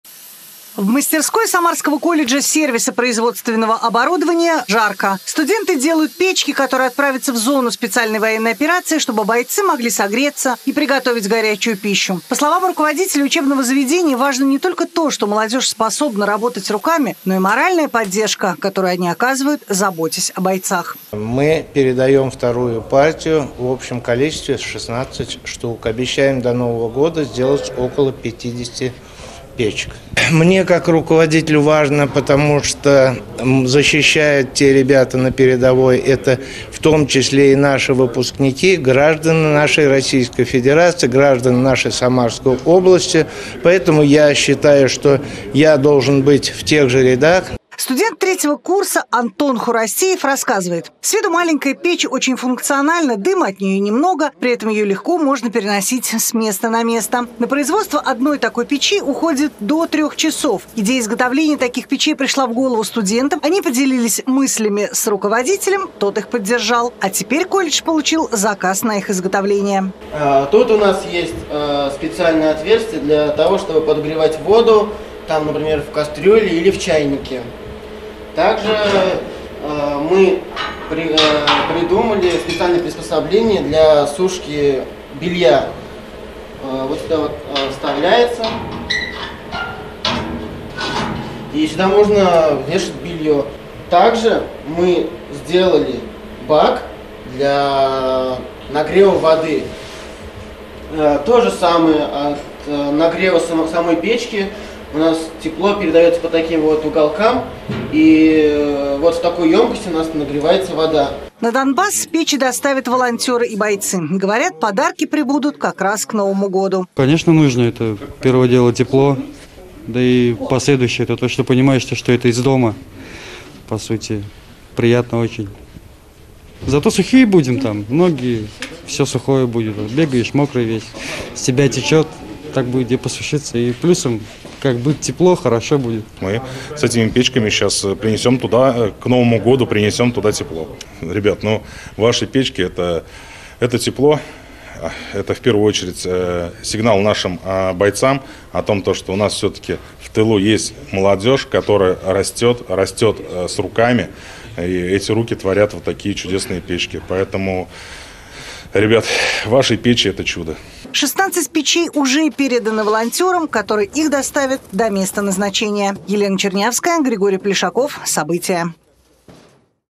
Самарские студенты передали 16 переносных печей для военнослужащих
Студенты Самарского колледжа изготовили железные мини-печи для участников специальной военной операции. Это уже вторая партия. О том, как преподавателям и учащимся пришла идея помочь, расскажет Елена Чернявская.